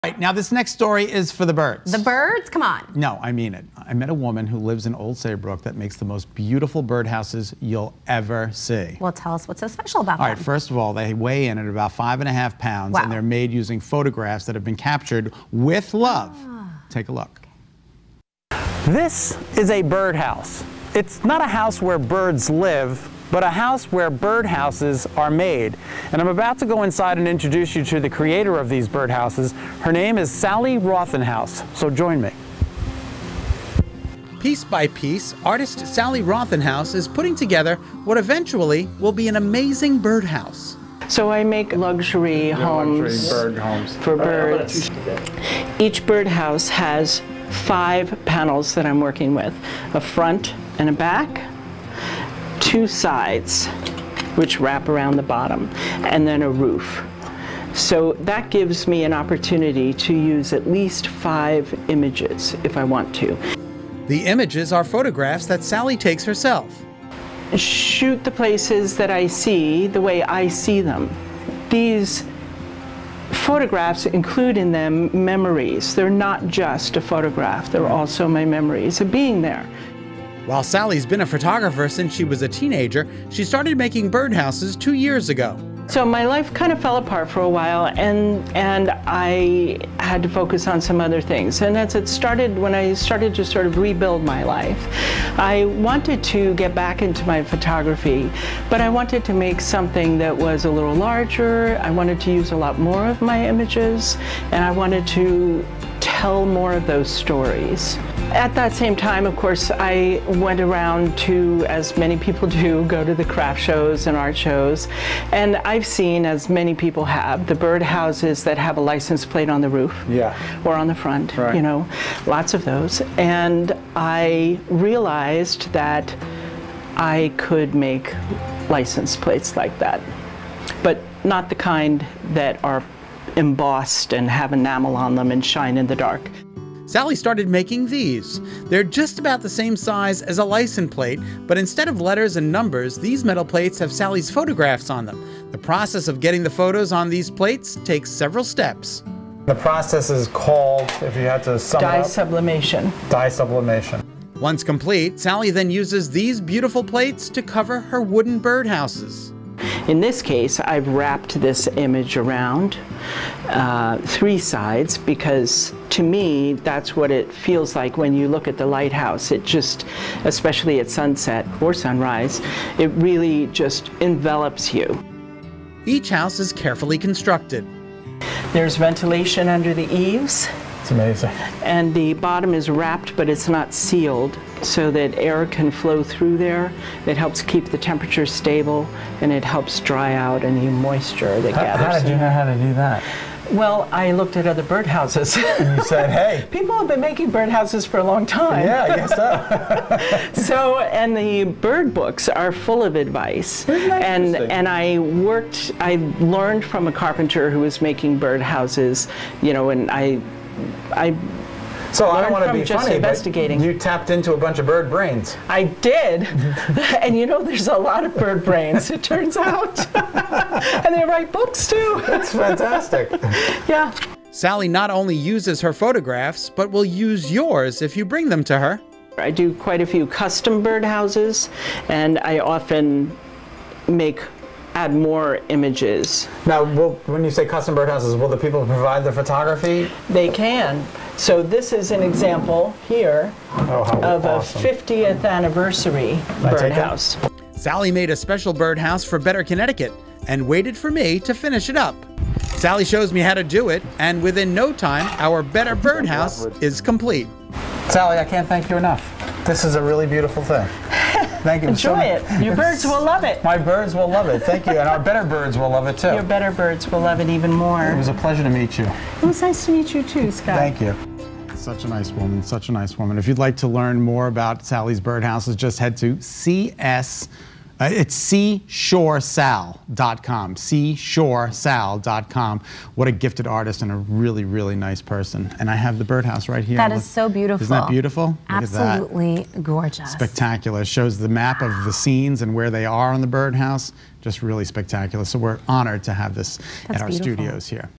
Check Out These Beautiful, Unique Birdhouses!
These are not your ordinary birdhouses, they weigh about 5.5 pounds and they're made using photographs that have been captured with love.  Sally Rothenhaus shows us how they're made.